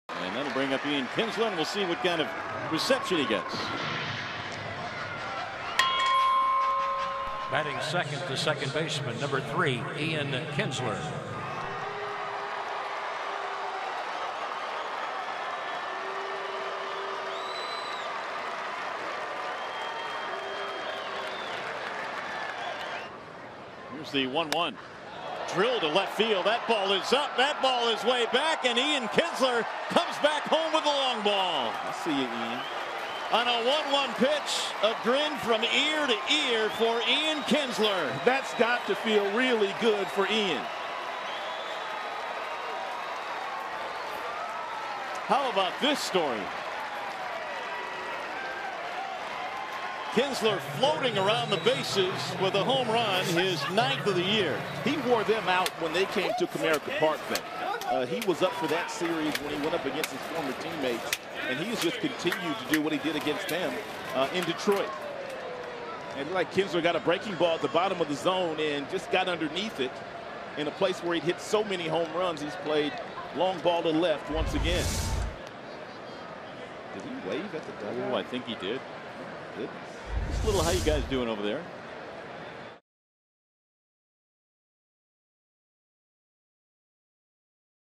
DET@TEX: Kinsler homers to left in return to Texas
6/24/14: In his return to Arlington, Ian Kinsler belts a solo home run to left and waves to his former teammates as he rounds the bases